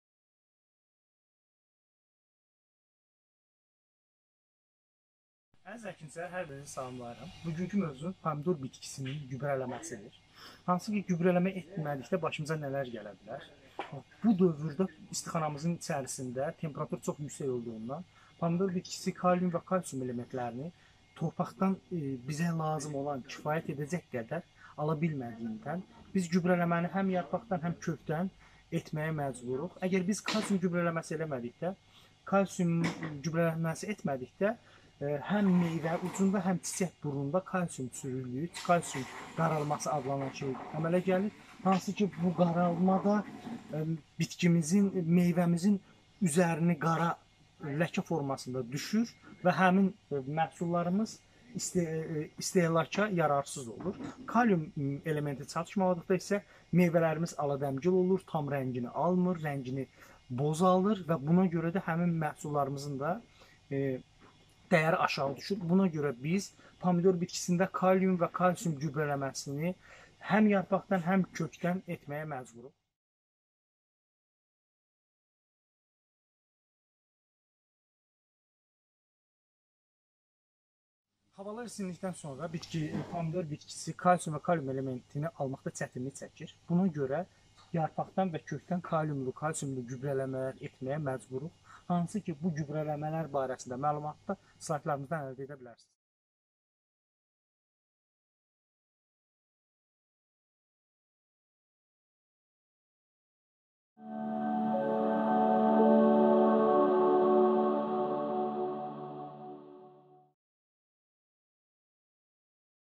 Pomidor Bitkisinin Gübrələnməsi
USAID Azərbaycan PSA Layihəsinin aqronomu Kərəm Nəcəfov vasitəsi ilə Azərbaycan tərəvəz istehsalçılarına pomidor bitkisinin gübrələnməsi ilə bağlı əsas məlumatları təqdim edir.

Video USAID Azərbaycan Özəl Sektorun İnkişafı (PSA) layihəsi çərçivəsində hazırlanıb.
Təqdimatçı: Kərəm Nəcəfov, PSA Layihəsinin Aqronomu